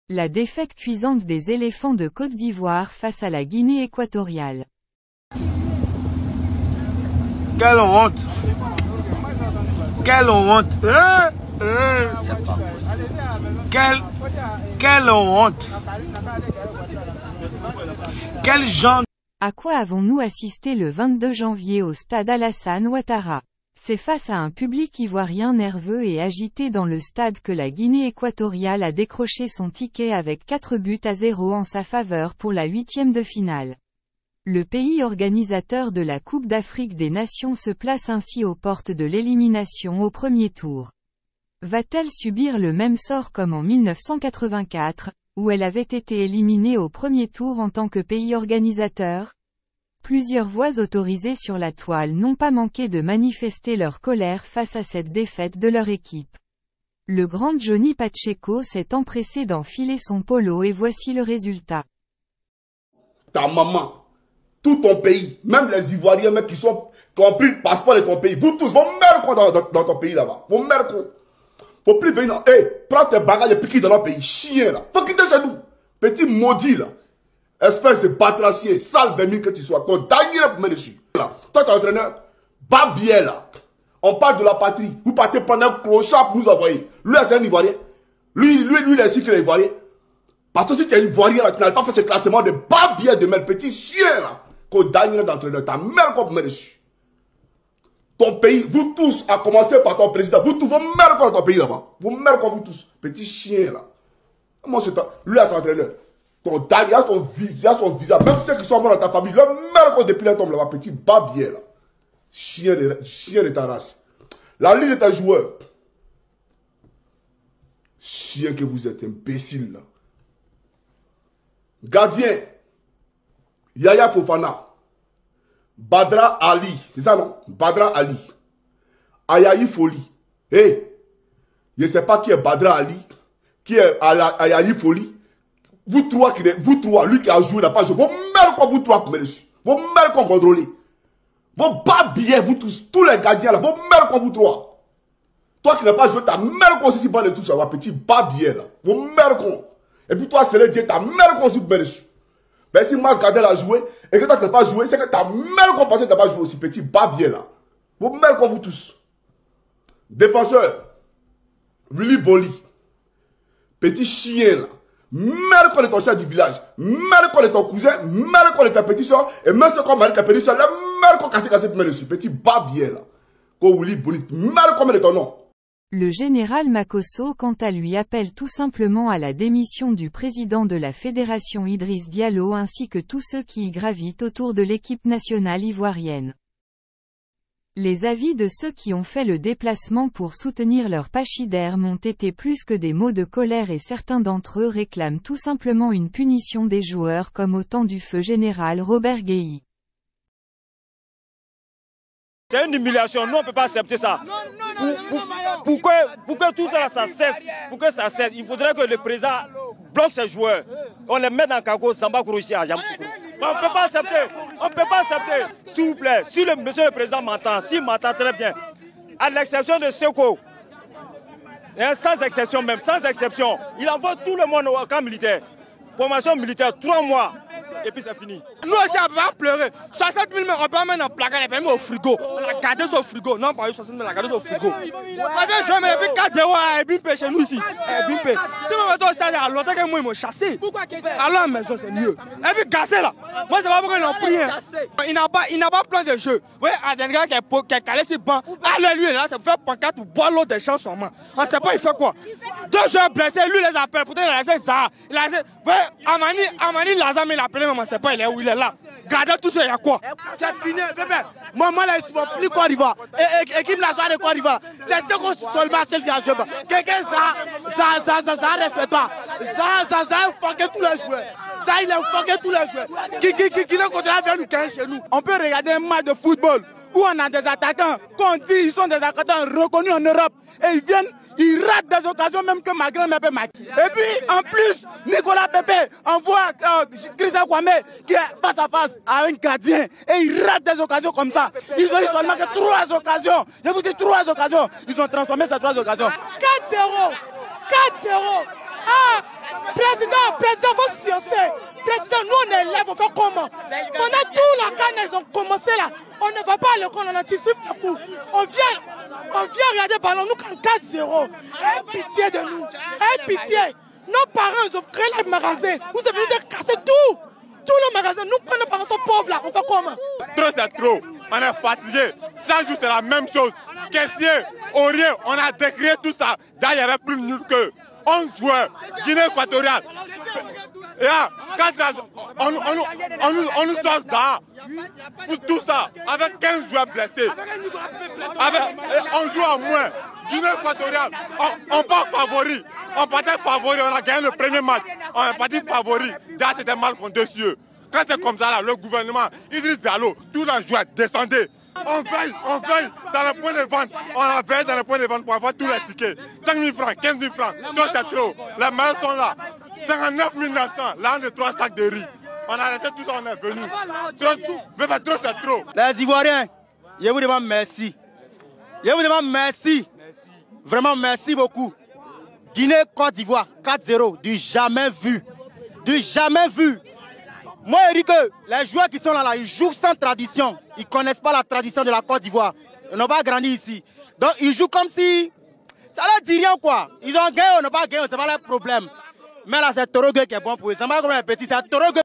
🚨🚨CAN 2024 | LA CHUTE DES ÉLÉPHANTS DE COTE D'IVOIRE - LA POPULATION EN C0LERE
Défaite cuisante de la cote d'ivoire .
la population en colère demande la démission du président de la fédération et de son entraineur.